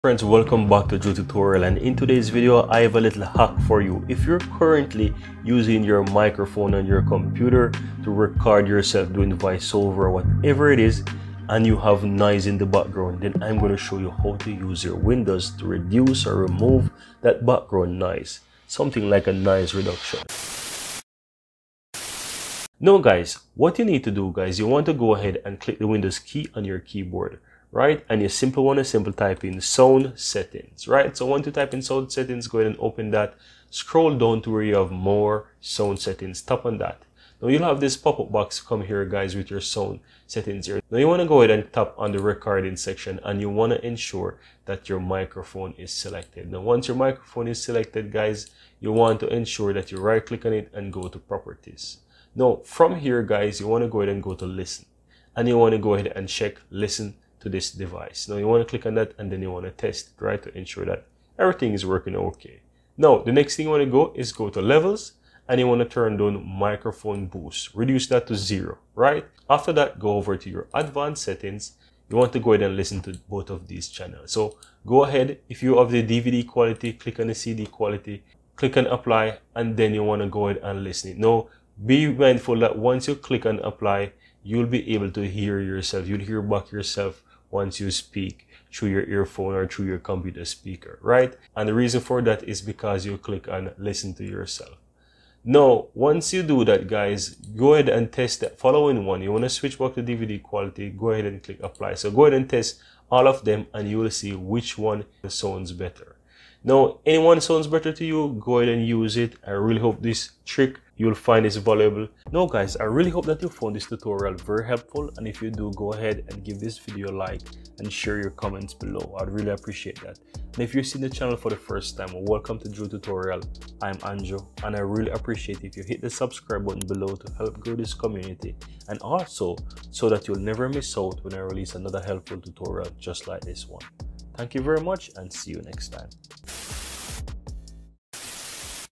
How To Remove Noise From Your Background in Windows 10/11
In this tutorial, I show you How To Remove Noise From Your Background in Windows 10/11